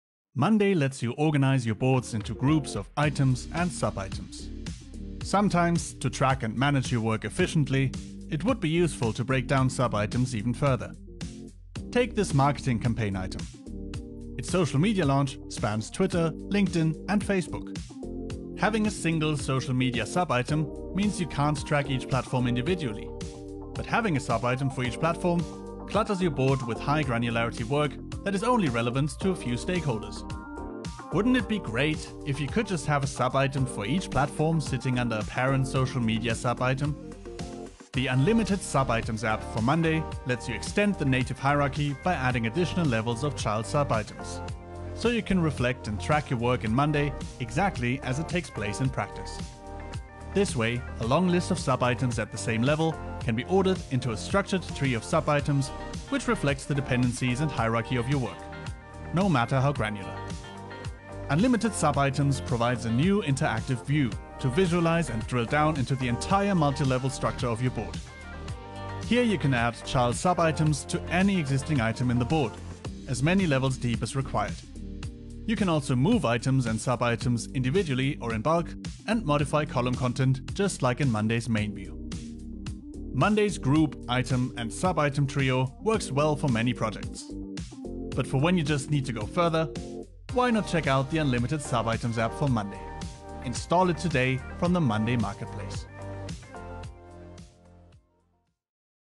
Take your monday.com boards to the next level with Unlimited Subitems
Unlimited Subitems lets you extend the native monday.com information hierarchy by adding as many levels of subitems as you need. A visual tree view lets you modify and drill down into your extended project structure, so you can accurately reflect the way you organize your work in monday.com

🎯 Accurately reflect your project in monday.com: large projects require tracking at multiple levels. Structure your tasks in their correct hierarchy by adding all the levels of subitems you need.

✅ Track multiple assignees, statuses and timeframes for a single monday.com subtask: project components often have more than one part. Break down any subtask into child components so you can manage the status, timing and team members working on each individual part.

👁 See everything at a glance: get a clear and simple overview of your extended board hierarchy to quickly identify specific problem areas, understand project component timings and determine who is working on what.

🎉 No more workarounds: tired of using checklists on subitems to reflect multiple subitem deliverables? Do it the right way and create individual subitems to capture and track all the information you need.